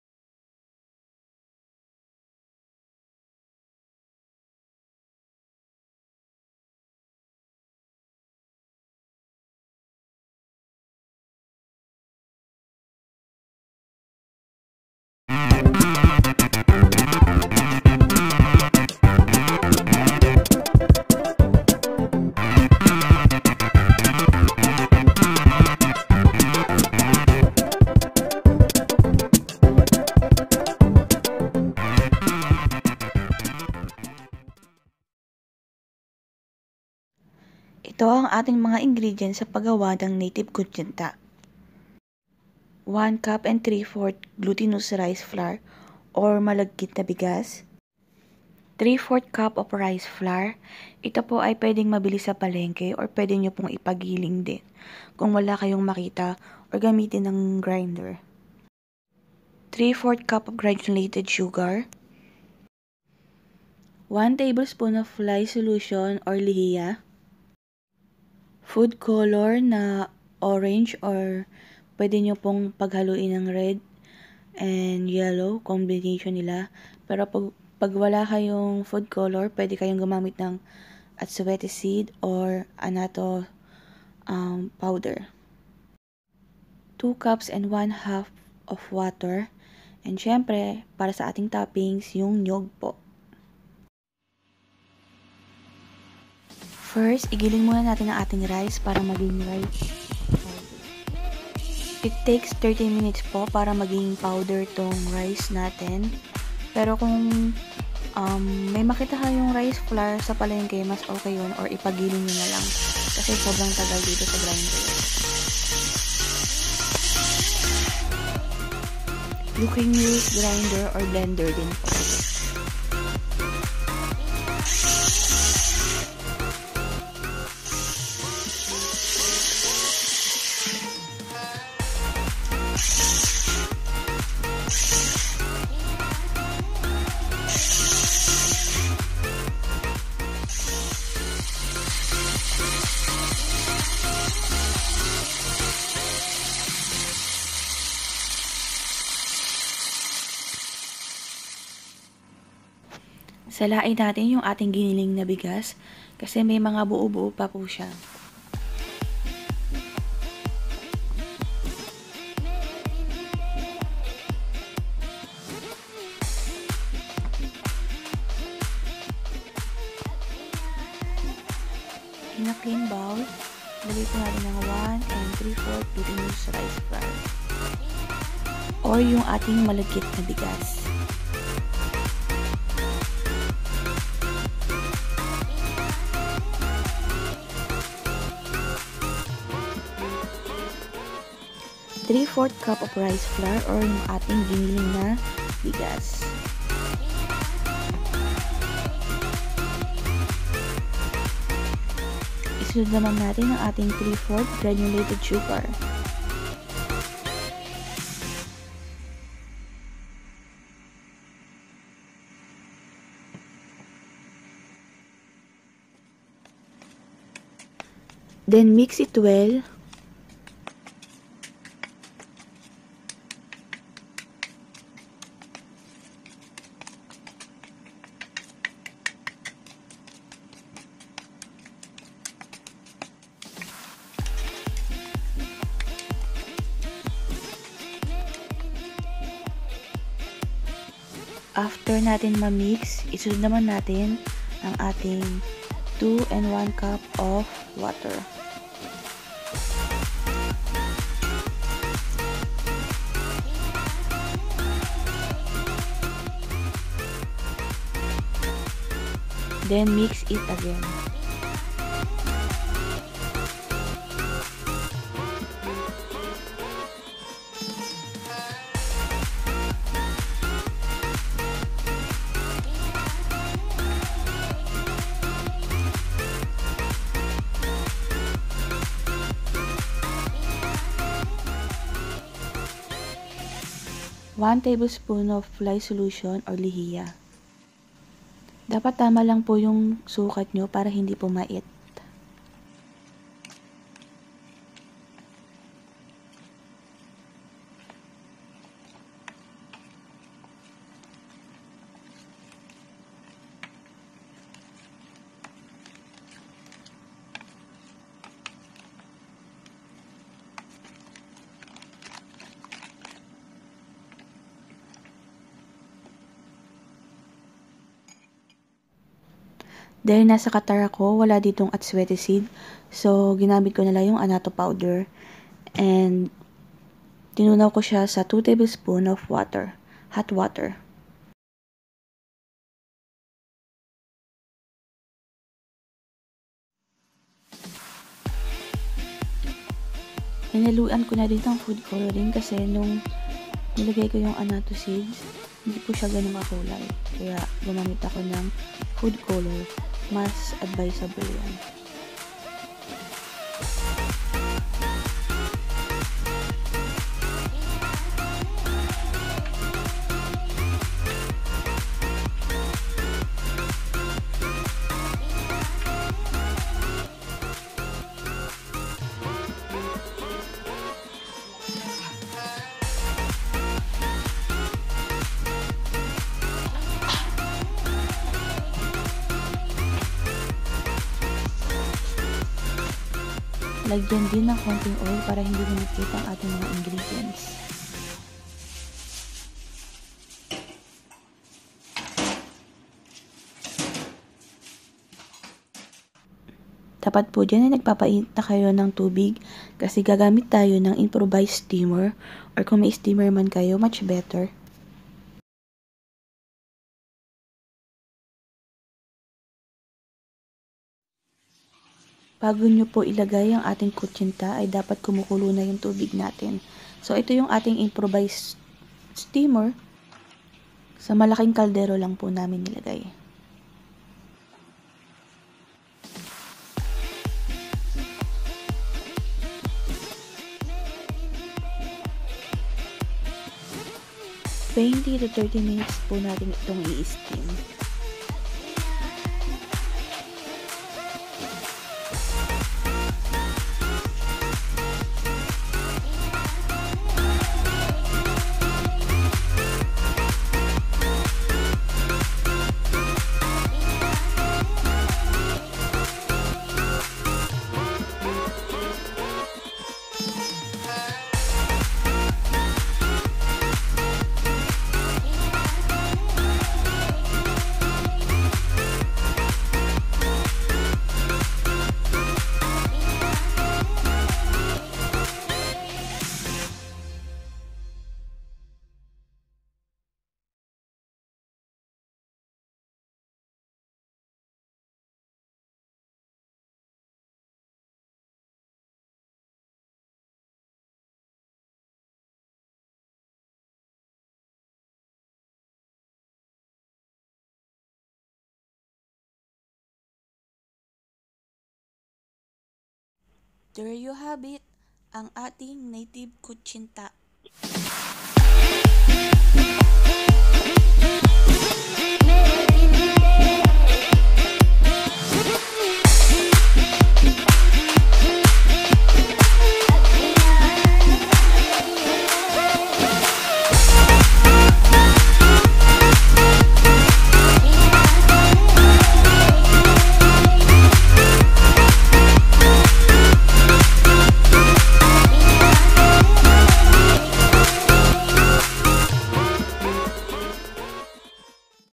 Kutsinta ll Native kutsinta ll How to make kutsinta
kutsinta is a type of steamed rice cake in the philippines. Made from the mixture of rice flour, sugar and lye water (lihiya) with yellow food coloring or annato extract.